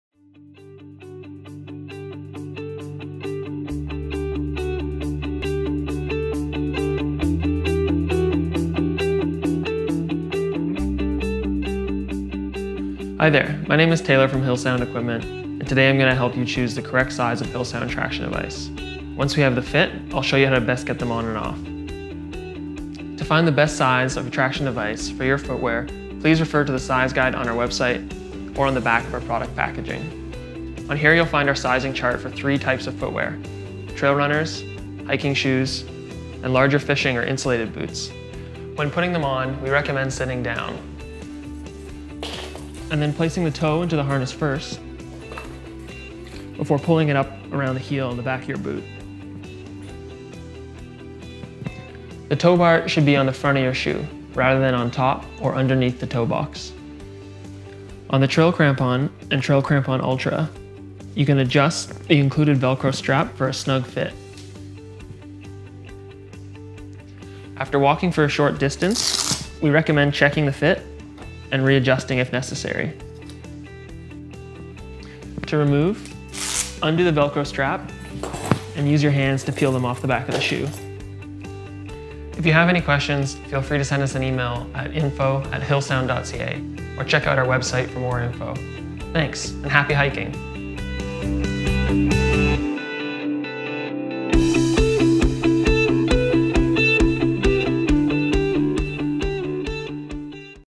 Crampon Fitting Tutorial - Hillsound Equipment
We created this helpful guide to demonstrate how to correctly size and fit Hillsound traction devices and crampons.